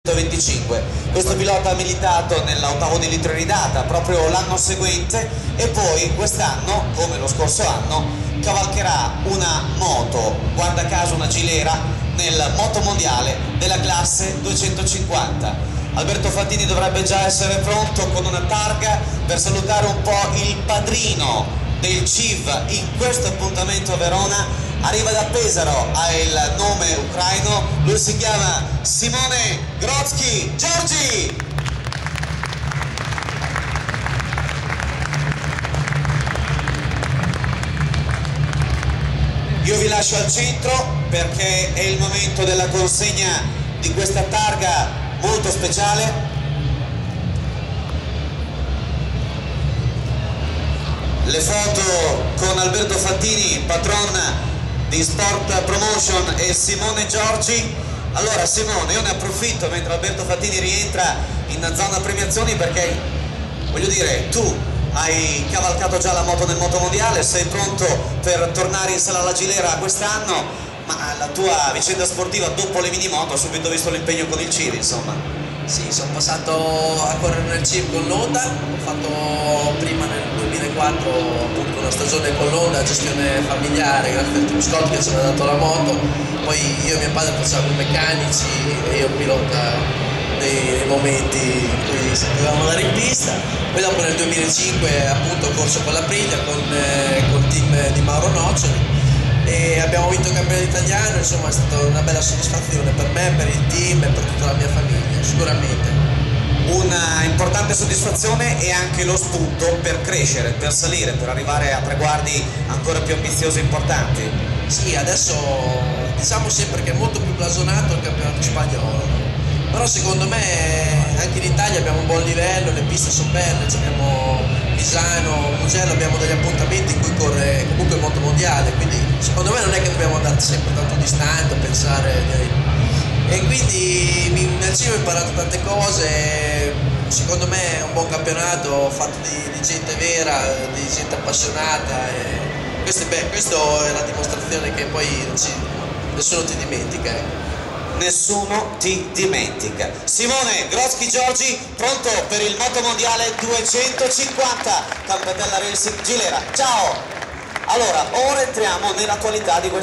premiazione
Simone Grotzkyj Giorgi (Motogp 250) viene premiato con una targa al Motor Bike Expo Verona 2009  dal C.I.V.